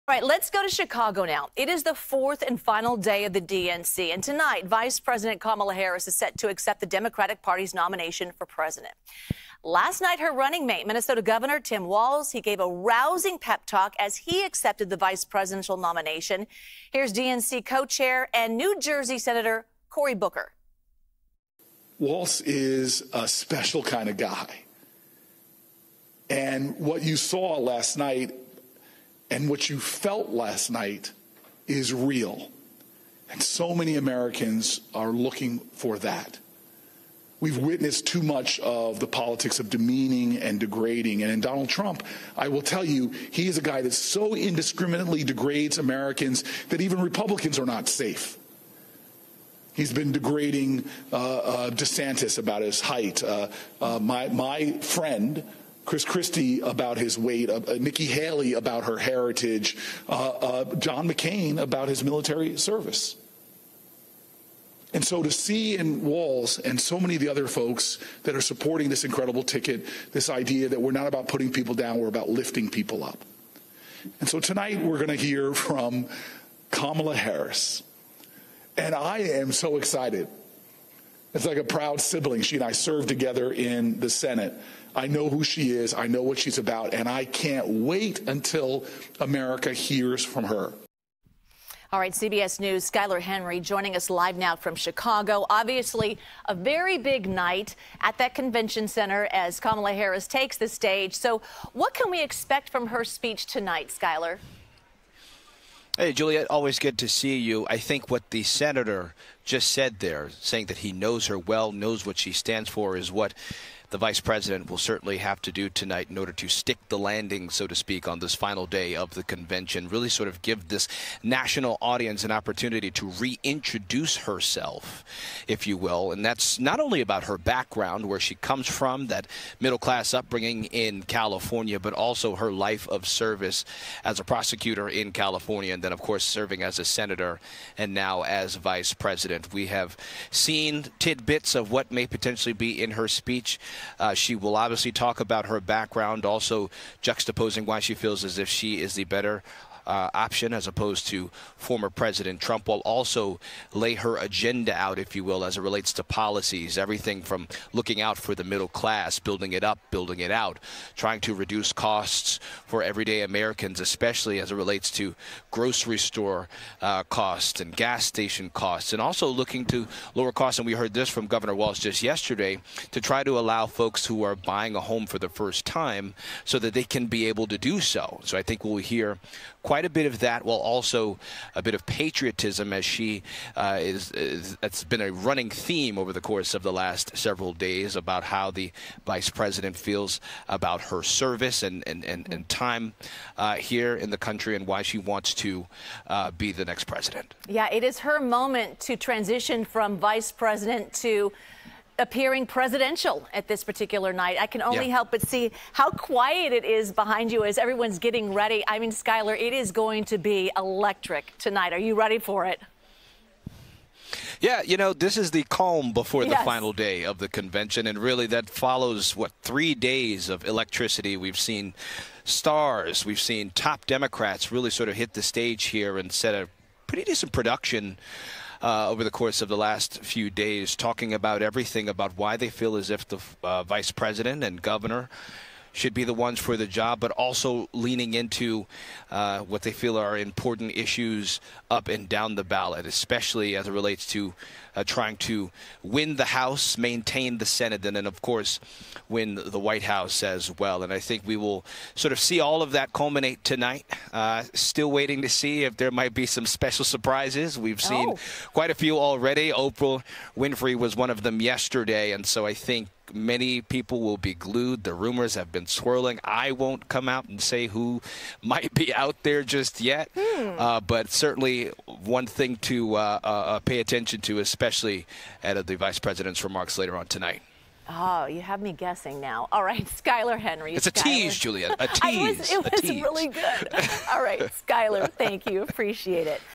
What's coming for Day 4 of DNC in Chicago
The final day of the Democratic National Convention will conclude with Vice President Kamala Harris' address in Chicago. This comes after Minnesota Gov. Tim Walz's speech accepting the nomination on Harris' ticket. CBS News' Skyler Henry reports as some speculate if a surprise is in store for the DNC.